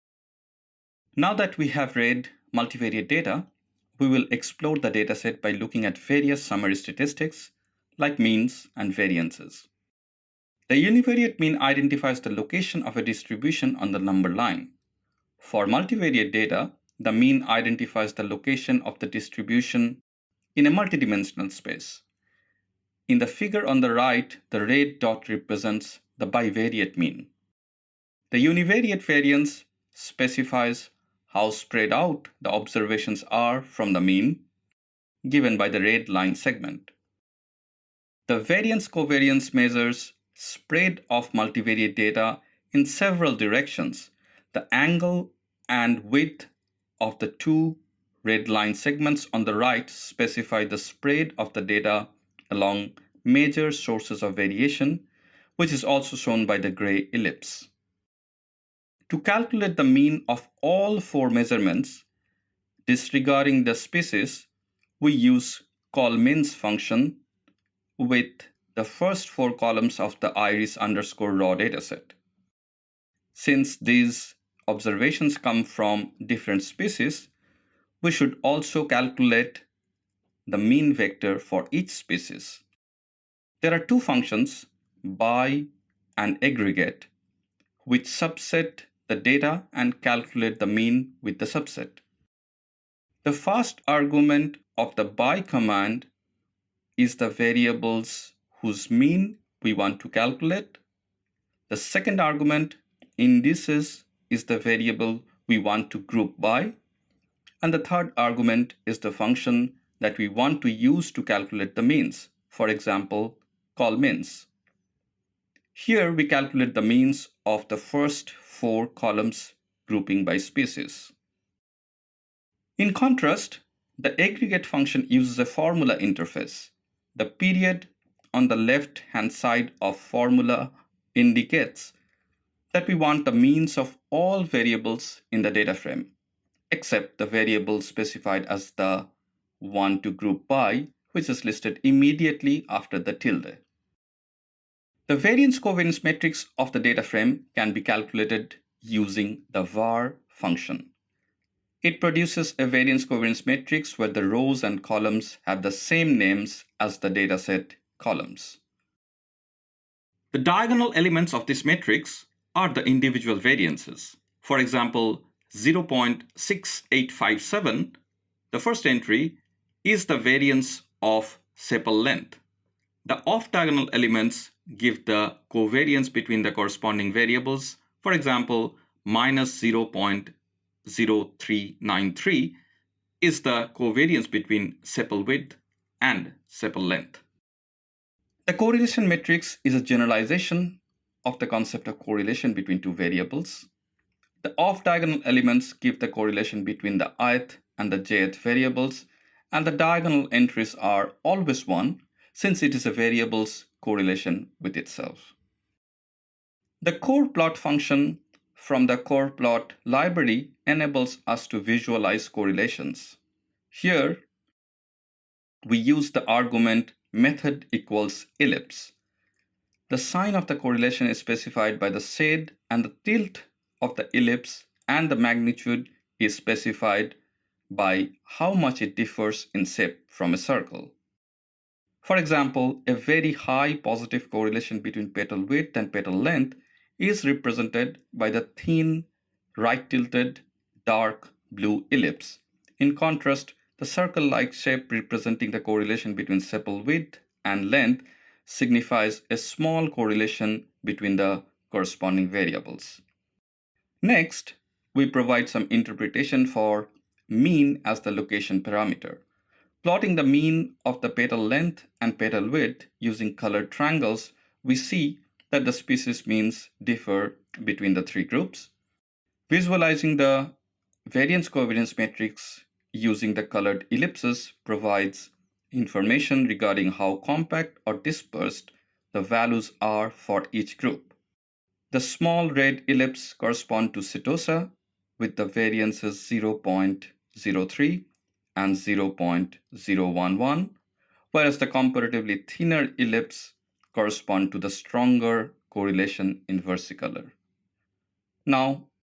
R Tutorial: Mean vector and variance-covariance matrix
Want to learn more? at your own pace. More than a video, you'll learn hands-on coding & quickly apply skills to your daily work.

Now that we have read multivariate data, we will explore the dataset by looking at various summary statistics, like means and variances.

The univariate mean identifies the location of a distribution on the number line. For multivariate data, the mean identifies the location of the distribution in a multidimensional space. In the figure on the right, the red dot represents the bivariate mean.

The univariate variance specifies how spread out the observations are from the mean, given by the red line segment.

The variance-covariance measures spread of multivariate data in several directions. The angle and width of the two red line segments on the right specify the spread of the data along major sources of variation, which is also shown by the gray ellipse.

To calculate the mean of all four measurements, disregarding the Species, we use the colMeans() function with the first four columns of the iris underscore raw dataset.

Since these observations come from different species, we should calculate the mean vector for each species. There are two functions, by() and aggregate(), which subset the data and calculate the mean within the subset.

The first argument of the by() command is the variables whose means we want to calculate, the second argument, INDICES, is the variable we want to group by, and the third argument is the function that we want to use to calculate the means, for example, colMeans(). Here we calculate the means of the first four columns, grouping by species.

In contrast, the aggregate() function uses a formula interface. The period on the left-hand side of the formula indicates that we want the means of all variables in the dataframe, except the variable specified as the one to group by, which is listed immediately after the tilde.

The variance-covariance matrix of the dataframe can be calculated using the var() function. It produces a variance-covariance matrix where the rows and columns have the same names as the dataset columns.

The diagonal elements of this matrix are the individual variances. For example, 0 point 6857, the first entry, is the variance of Sepal Length. The off-diagonal elements give the covariance between the corresponding variables, for example, minus 0 point 0393 is the covariance between Sepal width and Sepal length.

The correlation matrix is a generalization of the concept of correlation between two variables. The off-diagonal elements give the correlation between the ith and jth variables and the diagonal entries are always 1, since it is a variable's correlation with itself.

The corrplot() function from the corrplot library enables us to visualize correlations. Here, we used the argument method equals "ellipse". The sign of the correlation is specified by the shade and tilt of the ellipse and the magnitude is specified by how much it differs in shape from a circle. For example, a very high positive correlation between petal width and petal length is represented by the thin, right-tilted, dark blue ellipse. In contrast, the circle like shape representing the correlation between Sepal width and length signifies a small correlation between the corresponding variables.

Next, we provide some interpretation for mean as the location parameter. Plotting the mean of the petal length and petal width using colored triangles, we see that the species means differ between the three groups.

Visualizing the variance-covariance matrix using the colored ellipses provides information regarding how compact or dispersed the values are for each group. The small red ellipse corresponds to setosa with the variances 0 point 03 and 0 point 011, whereas the comparatively thinner ellipse corresponds to the stronger correlation in versicolor.